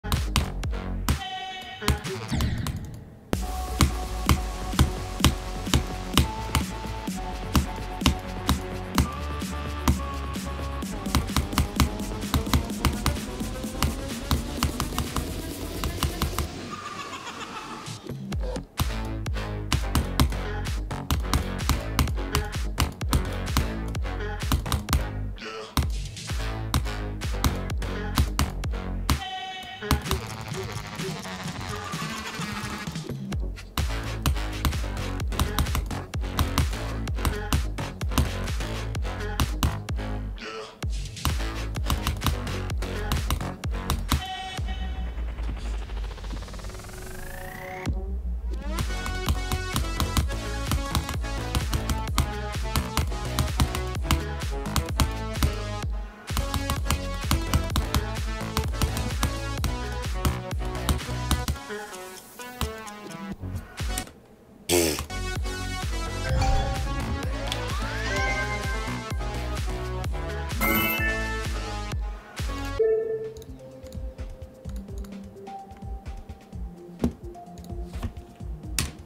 Extinction 100%
Fun level fluke from 50. 153 attempts